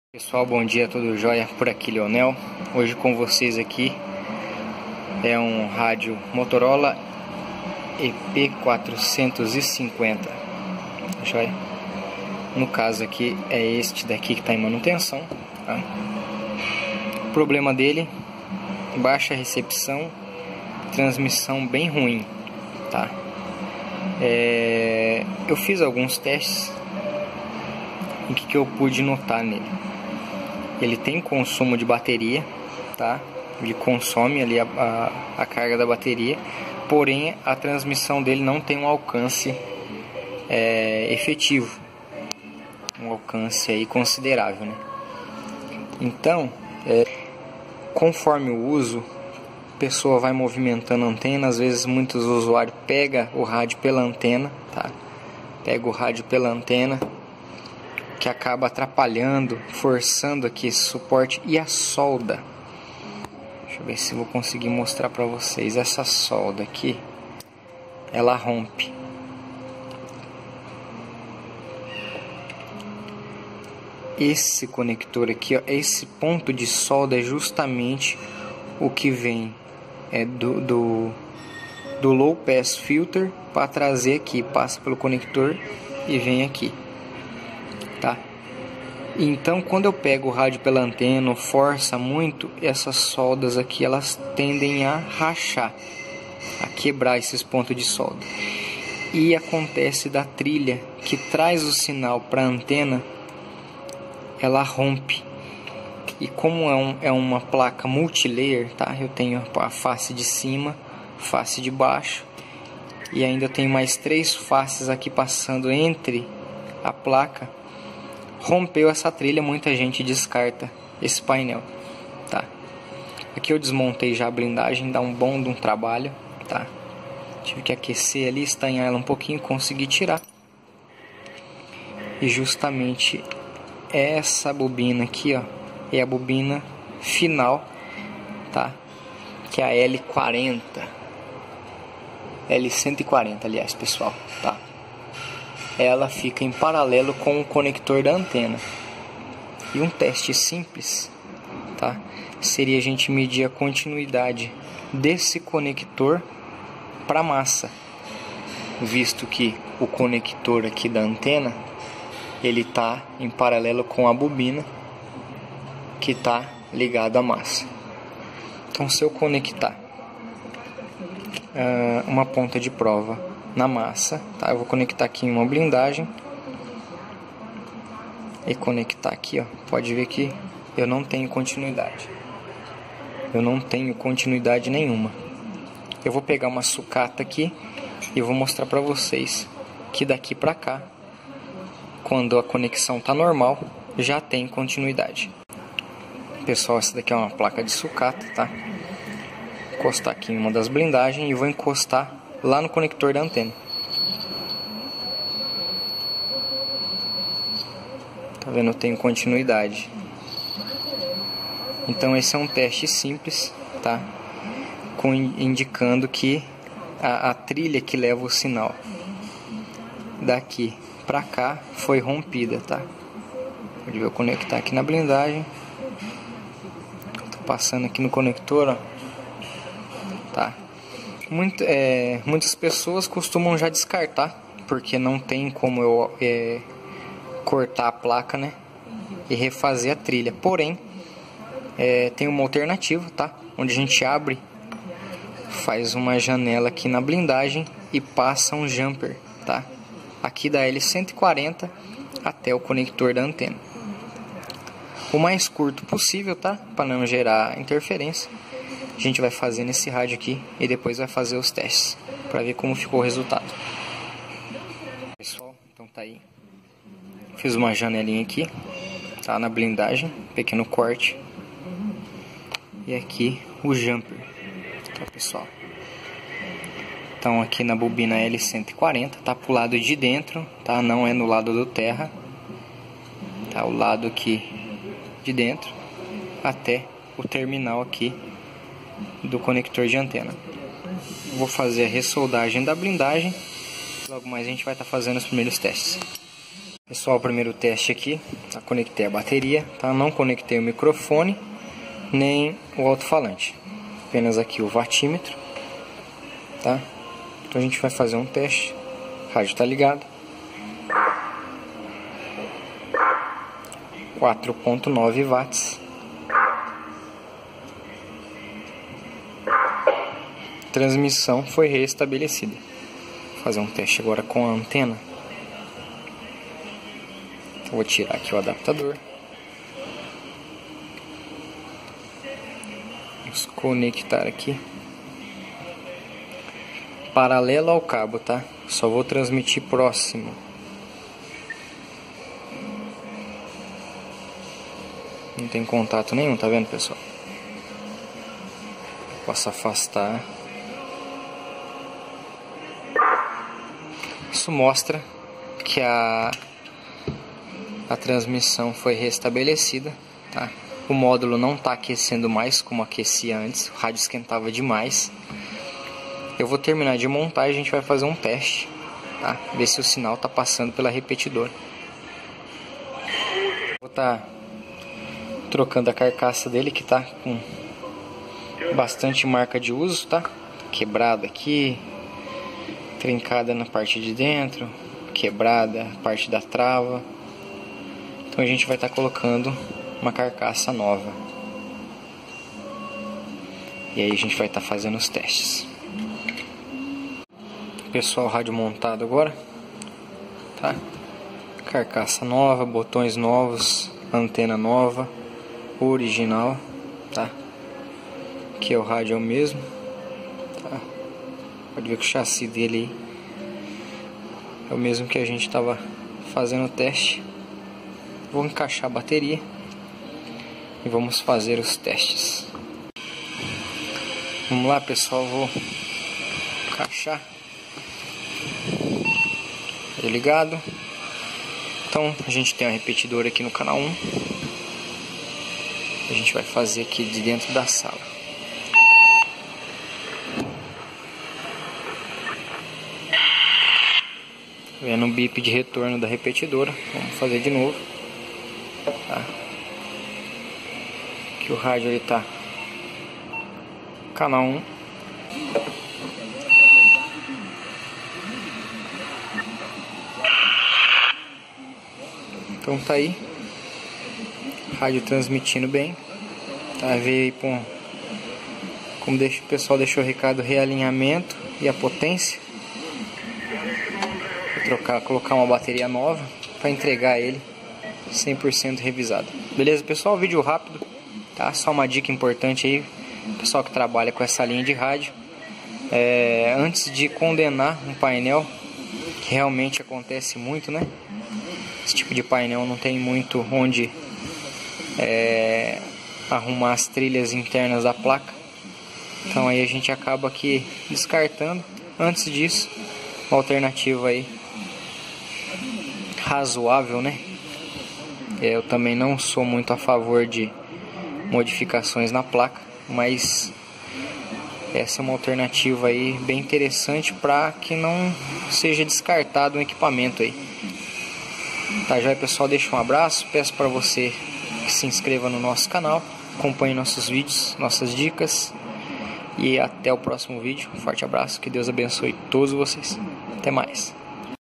Recepção e transmissão rádio Motorola EP 450
Neste vídeo veremos como fazer a manutenção do rádio Motorola EP 450, uma dica importante sobre como reparar um defeito corriqueiro neste modelo de rádio.